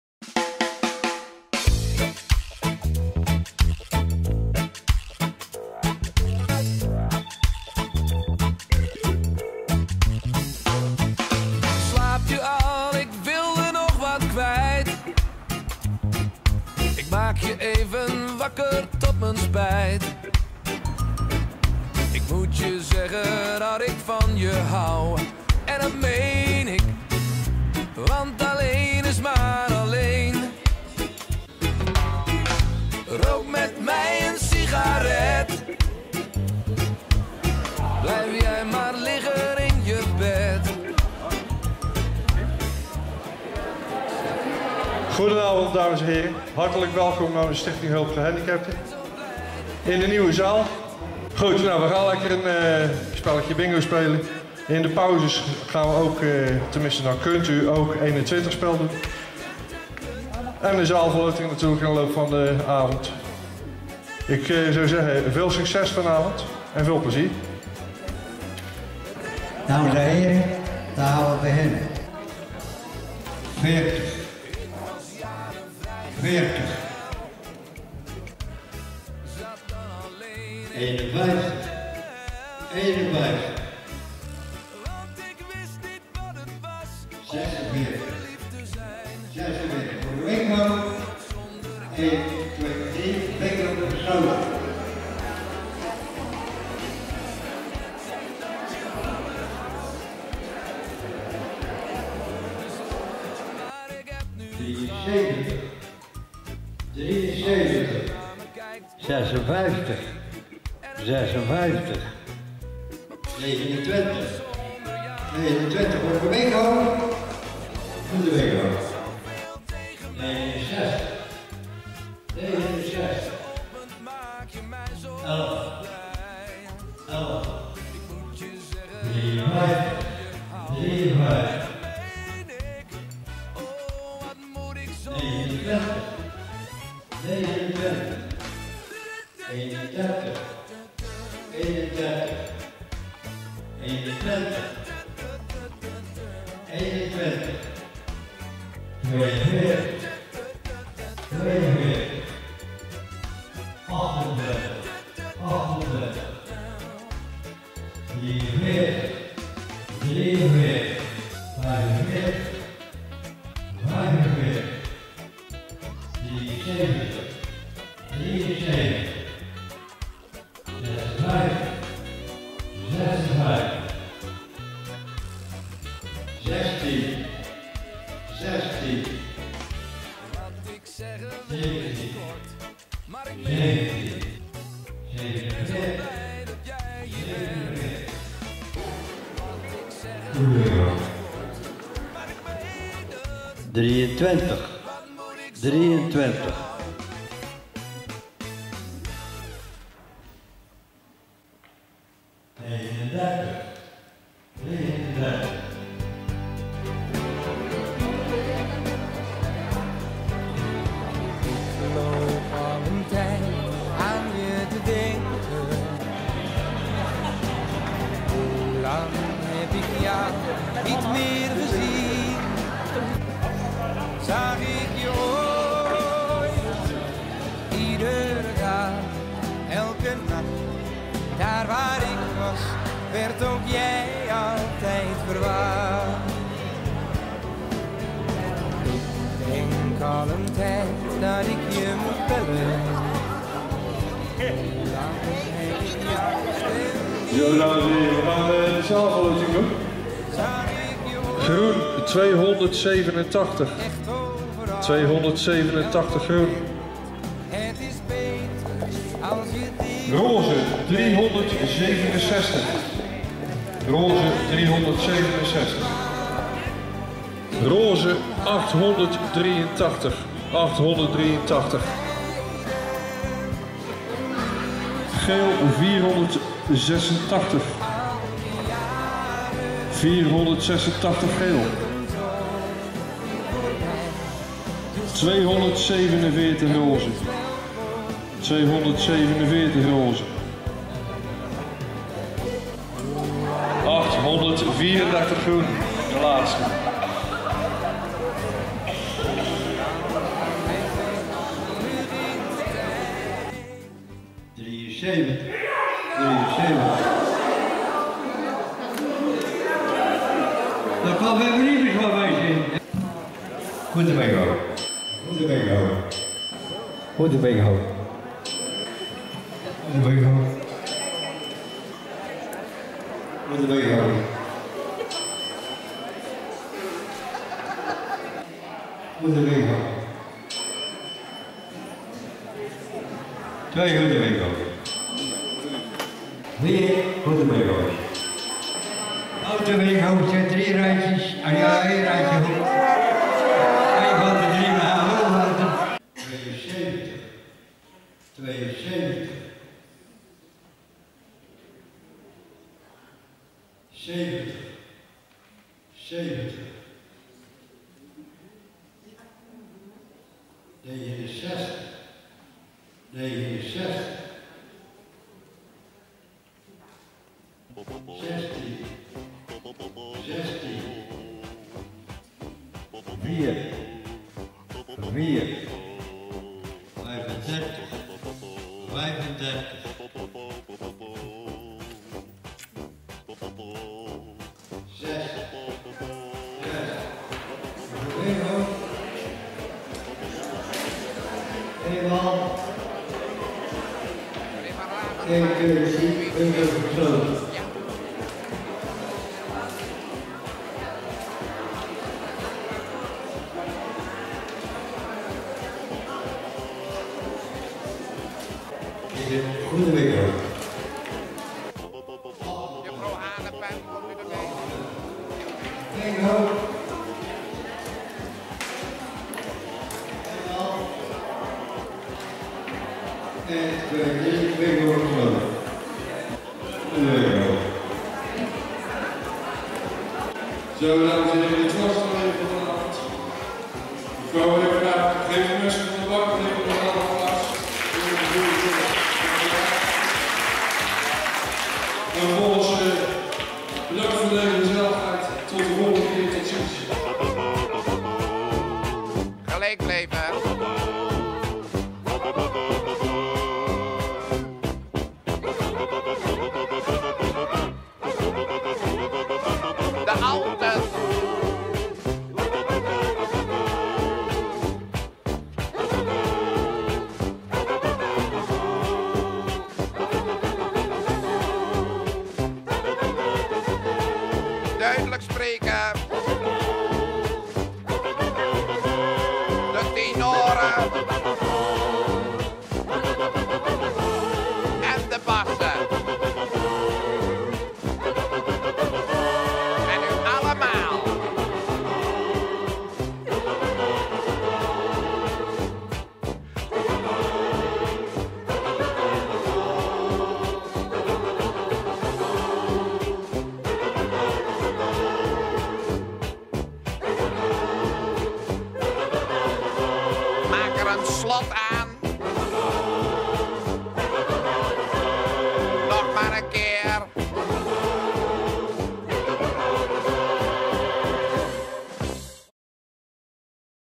Bingo in het nieuwe Goese Lyceum
Zaterdag 18 oktober 2014 waren wij voor het eerst welkom in het nieuwe gebouw van het Goese Lyceum. Een splinternieuwe aula was het toneel voor de grote najaarsbingo, waar ruim 100 personen zich hebben ingezet voor het goede doel. Bekijk hier het videoverslag van deze bijzondere avond.